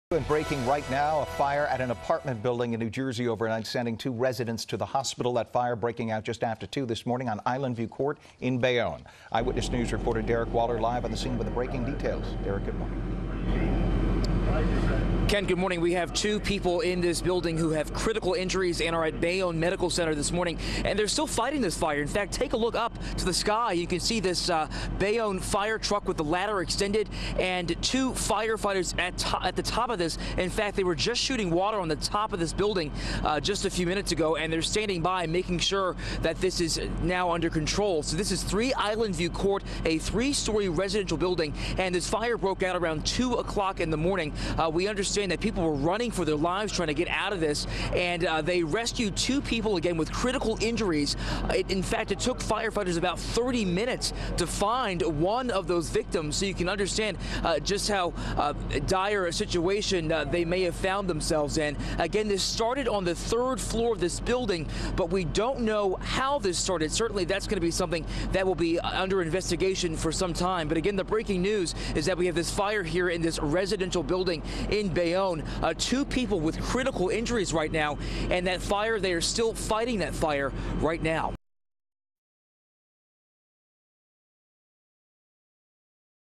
1 dead, 1 critical after fire burns through apartment building in Bayonne, NJ
A man in his 70s was killed and another person critically injured in a second alarm fire that burned through an apartment building in Bayonne, New Jersey, leaving the residents of 54 apartments homeless.

The blaze was reported just after 2 a.m. Friday at 3 Islandview Court.

Firefighters encountered heavy fire on the third floor when they arrived at the three-story building.

The fire quickly went to a second alarm.

After knocking down the initial flames, crews spent several hours battling hot spots that continued to flare throughout the third floor.

Eyewitness News is told firefighters initially pulled one resident from the building in unknown condition.

They received multiple reports of an unaccounted for resident and kept searching.

NEW HERE? –